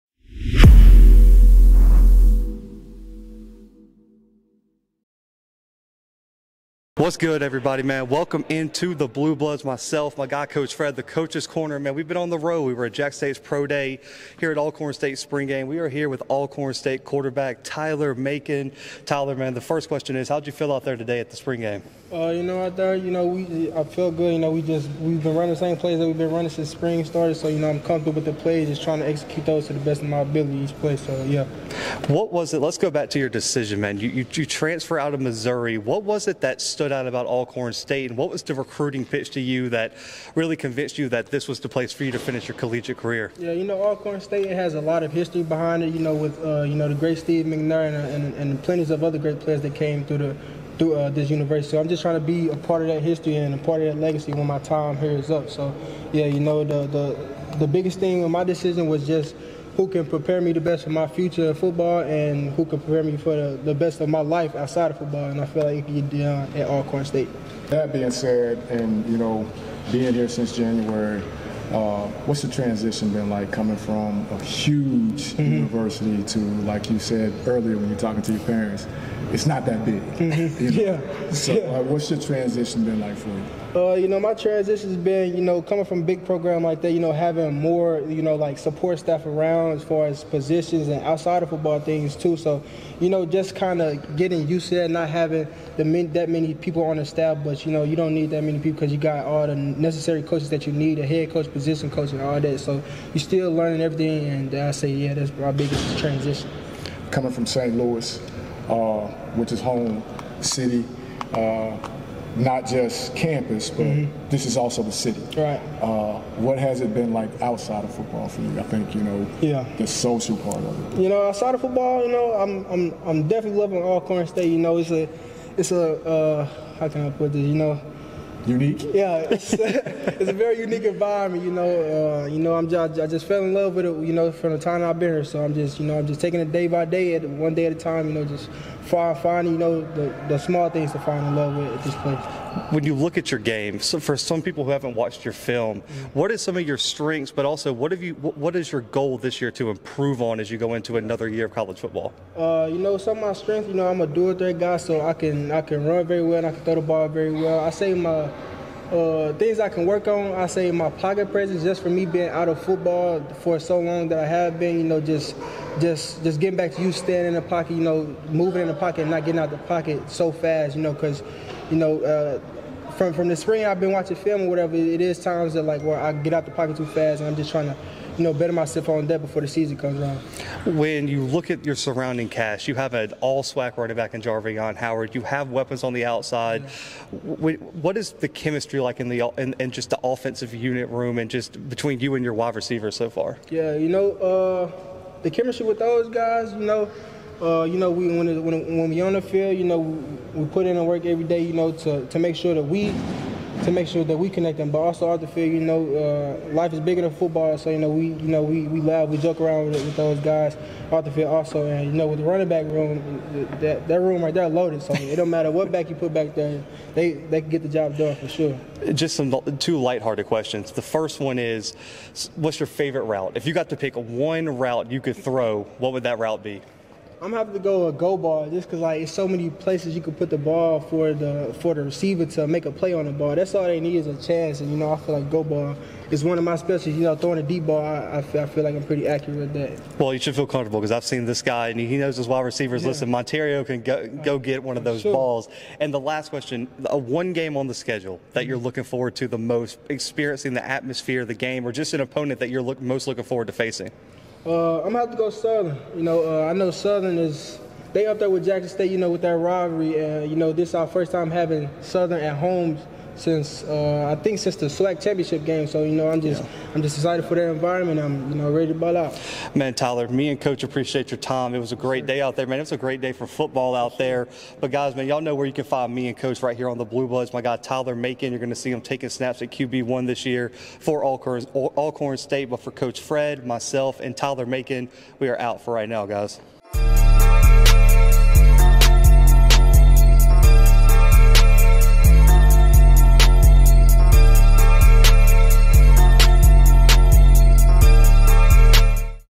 2023 Alcorn State Spring Game: Tyler Macon (Alcorn State QB) Interview | The Bluebloods
The Bluebloods caught up with Alcorn State QB Tyler Macon at the Alcorn State Spring Game in Lorman, MS. Stay tuned for more content from Lorman, MS right here on The Bluebloods throughout the week!